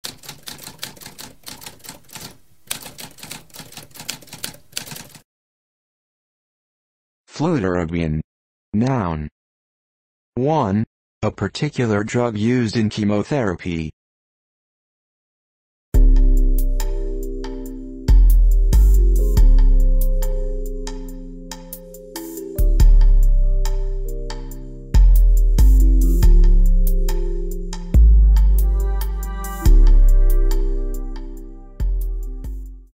What does fludarabine mean?
A spoken definition of fludarabine.

Intro Sound:
Typewriter - Tamskp